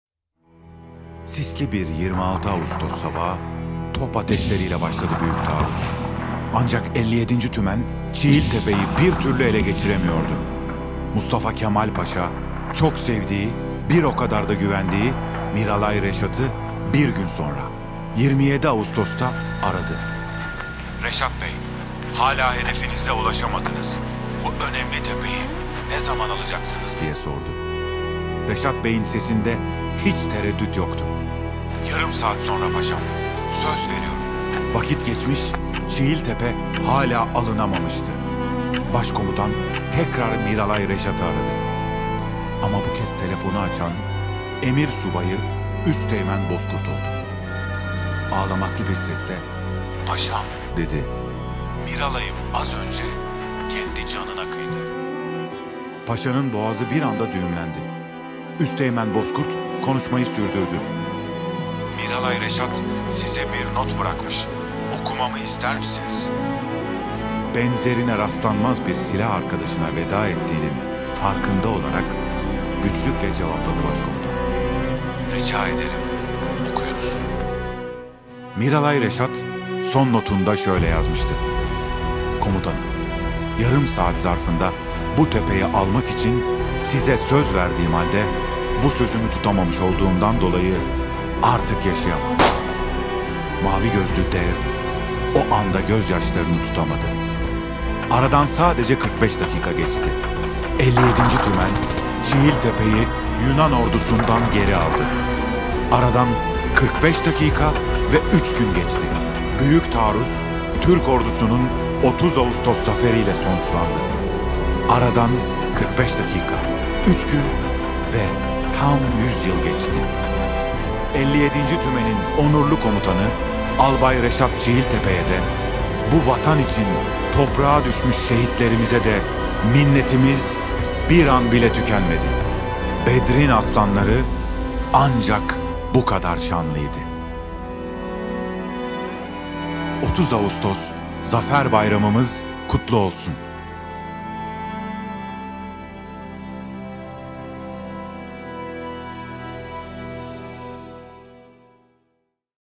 Aradan 100 değil, 1000 yıl geçse de bu vatan uğruna toprağa düşmüş askerimize minnetimiz tükenmez.
Aradan 100 değil, 1000 yıl geçse de bu vatan uğruna toprağa düşmüş askerimize minnetimiz tükenmez. 30 Ağustos Zaferimizin 100’üncü yılı kutlu olsun.